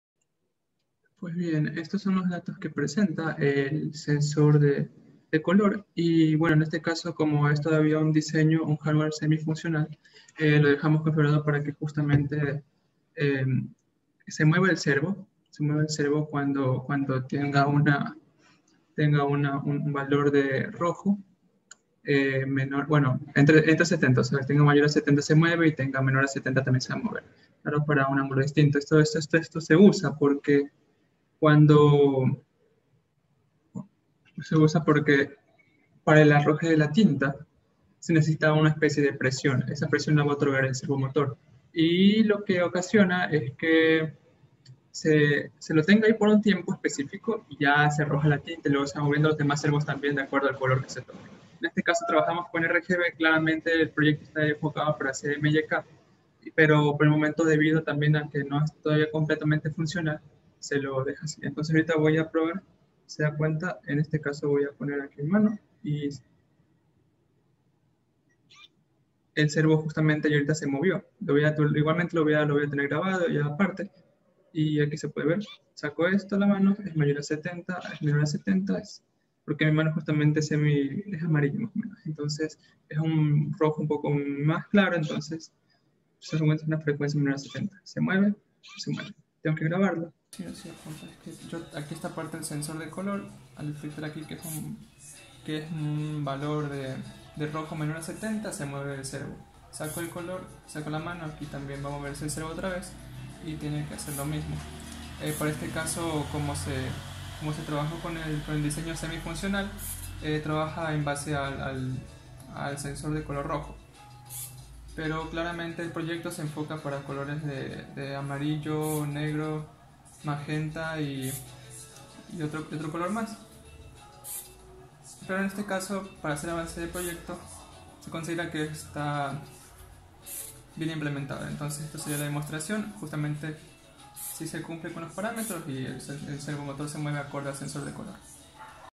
NODEMCU Color Sensor and Servo
Diseño semifuncional del proyecto mezcladora de colores.
Con un servo representativo y un sensor de color.
Avance 2 de la materia de Sistemas Telemáticos.
Hace falta la implementación de la conexión Wifi con el dispositivo.